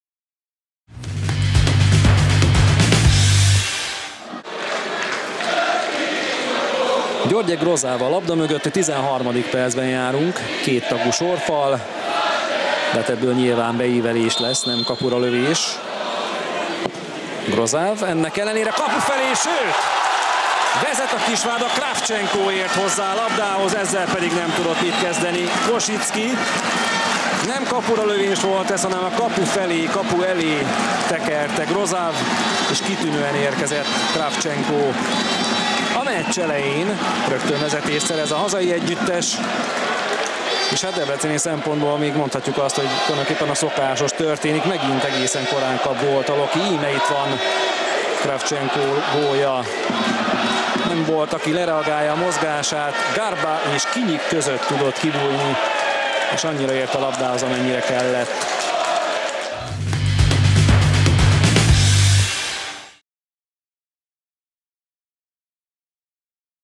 Anton Kravchenko gólja a Kisvárda Master Good – DVSC mérkőzésen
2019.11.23. OTP Bank Liga, 13. forduló
Kisvárda Master Good – DVSC | 1-0 | (1-0) |
Anton Kravchenko gólja